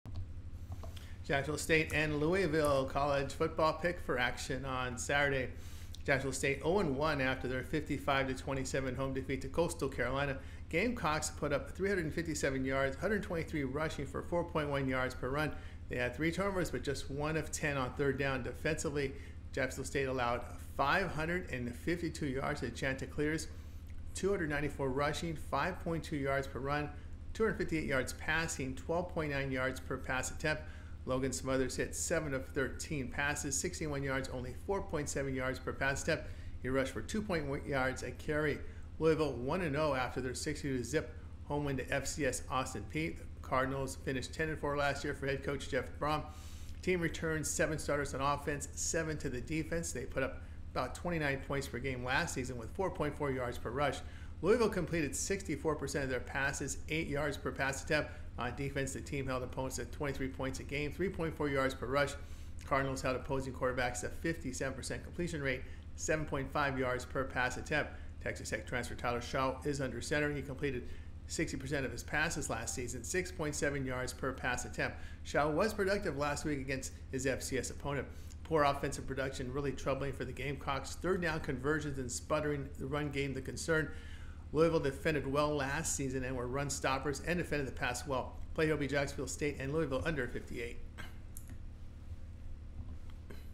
Jacksonville St vs. Louisville Pick 9/7/2024 NCAAF Week 2 Predictions
9/7/2024 Jacksonville St vs. Louisville  NCAAF Picks & Prediction by Tony Tellez,